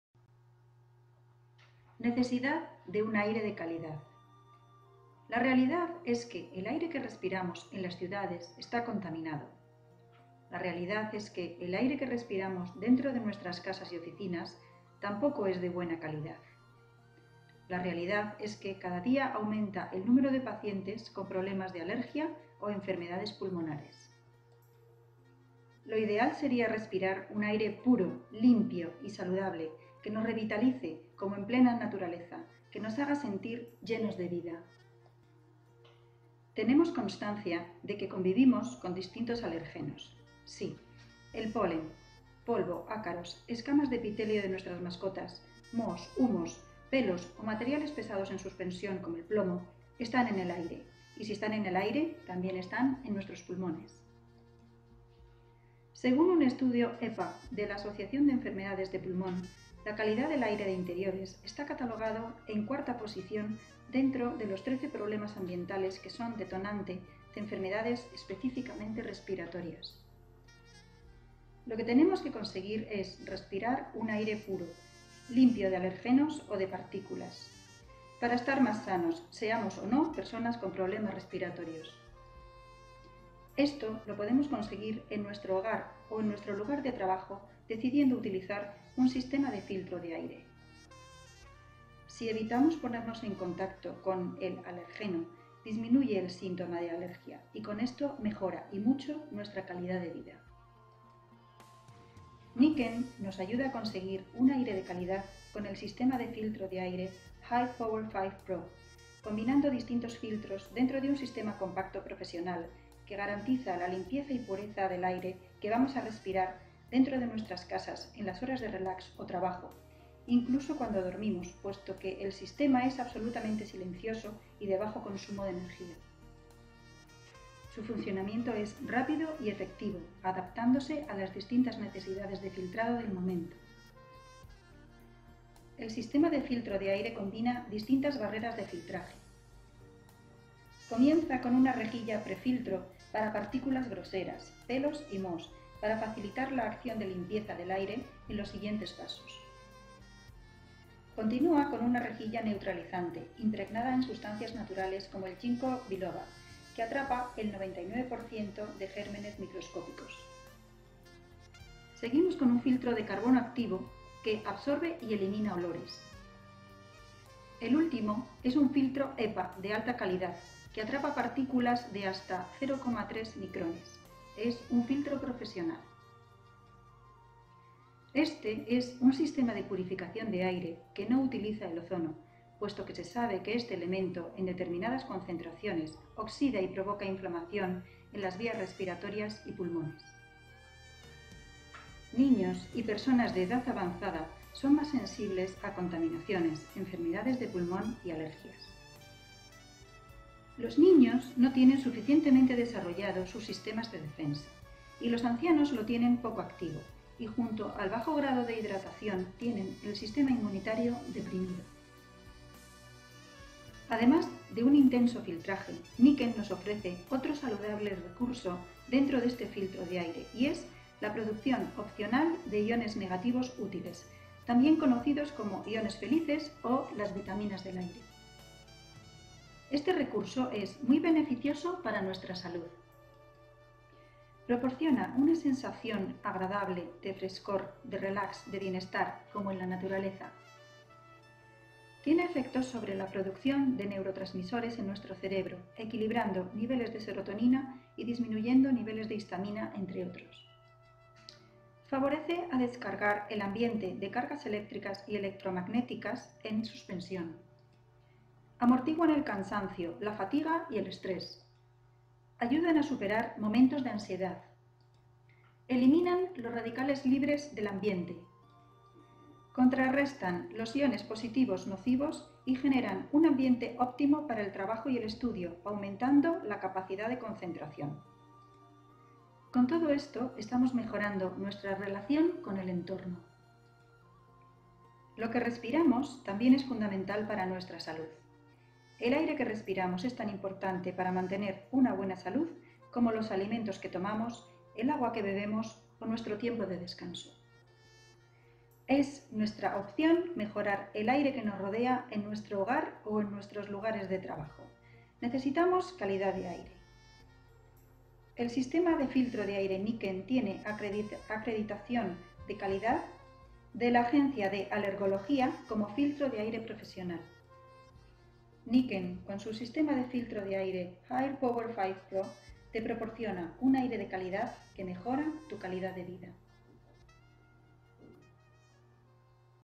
Aire de calidad! In Spanish
A Qualidade do Ar em Espanha e seus Maleficios!